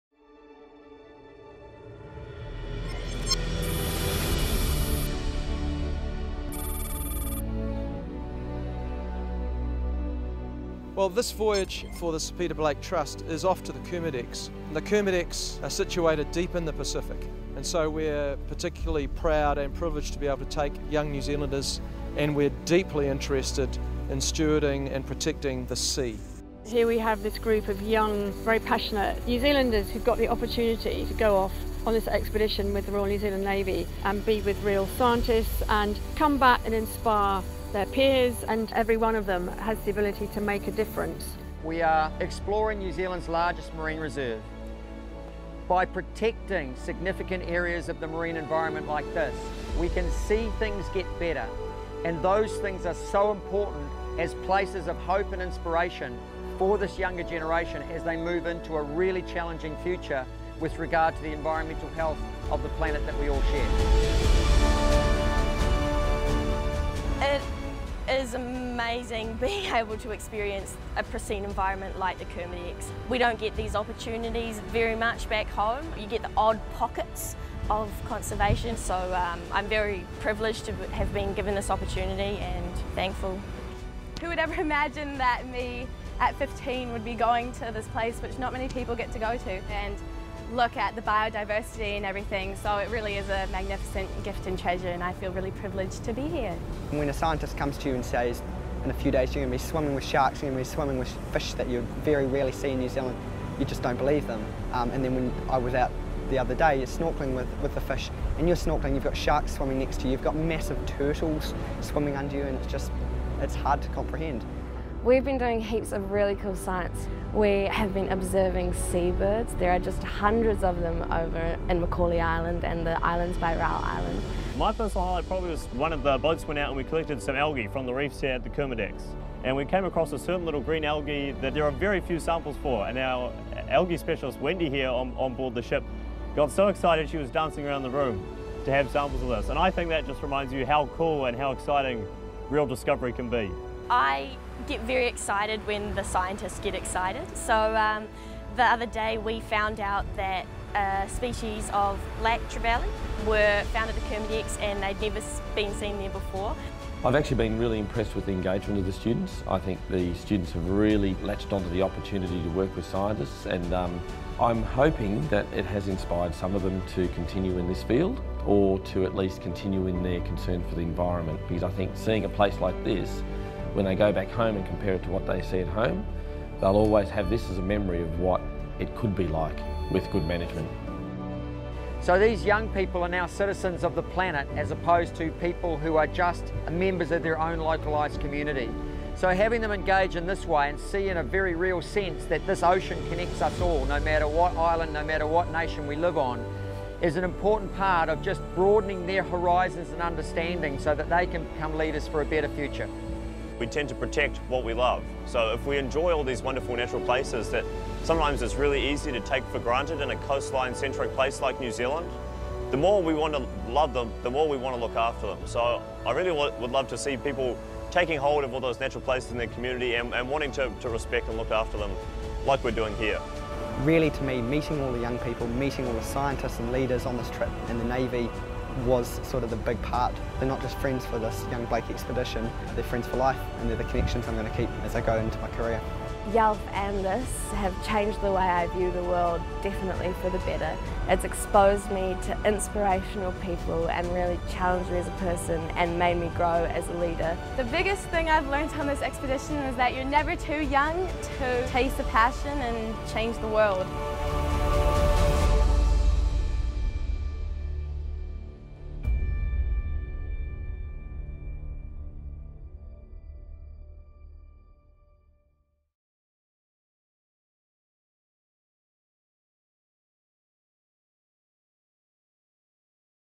BLAKE Expeditions 2018 - Kermadec Islands
20 student voyagers, and 23 crew members, embarked on an expedition to the Kermadec Islands on board HMNZS Canterbury. One of the most remote and biodiverse spots on the planet, this untouched region has much to teach New Zealand and the world around climate change and ocean health.